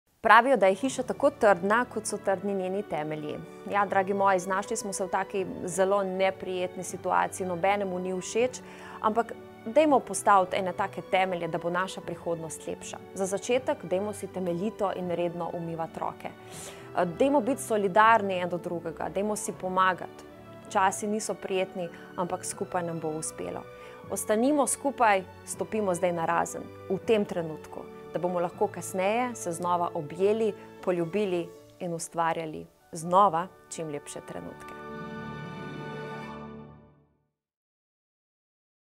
JANA MORELJ: ''Ostanimo skupaj, stopimo narazen, da se bomo kasneje lahko objeli''
Voditelji Radia 1 so stopili skupaj, a hkrati ostali narazen. Pozivajo te, da ostaneš doma in predvsem ostaneš zdrav! Želijo ti sporočiti, da bo še vse dobro in da bomo skupaj zmogli ... USPELO NAM BO!